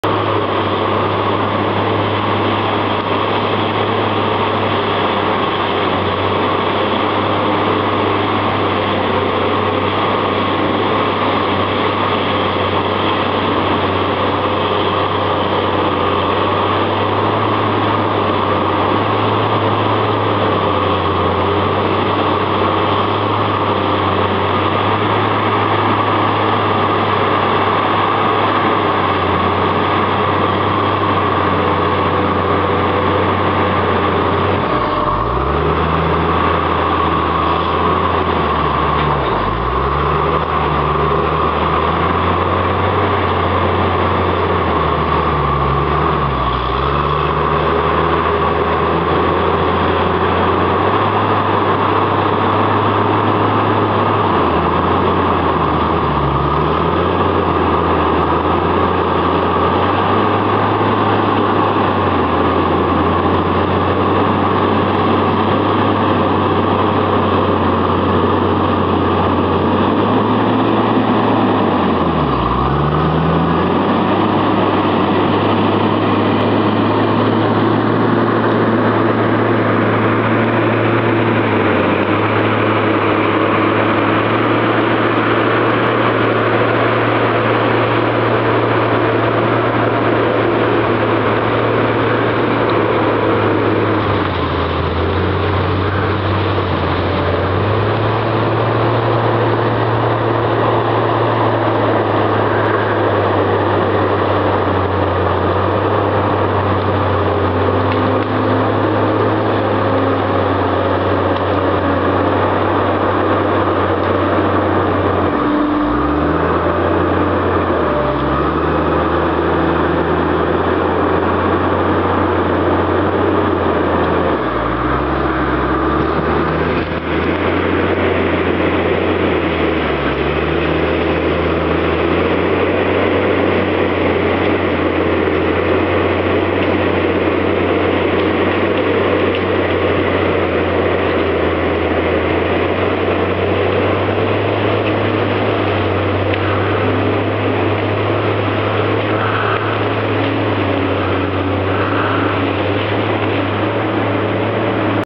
The power of 300 horses pull a hitch up over a hill and to the landing.
In days gone by, loggers never would have taken this direct rout. We would have gone with the lay of the land and looked for a rout the horses could pull a single log at a time.